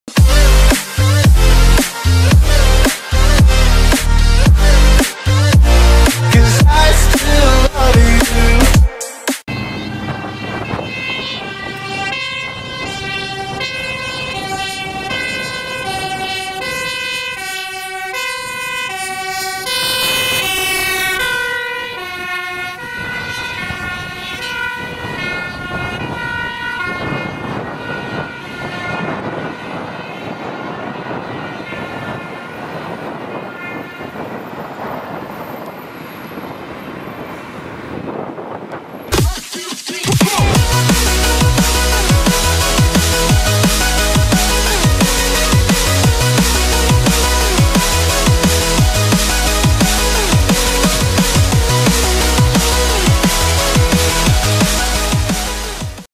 Oude Tankautospuit 17-4131 Brandweer Stellendam met spoed onderweg naar persoon te water in Ouddorp
In deze video: Oude Tankautospuit 17-4131 Brandweer Stellendam met spoed onderweg naar persoon te water in Ouddorp

Voertuigen in deze video:
- Oude Tankautospuit 17-4131 Brandweer Stellendam

Deze video mag op geen enkele manier gebruikt worden zonder toestemming van de eigenaar, op alle video's rust copyright.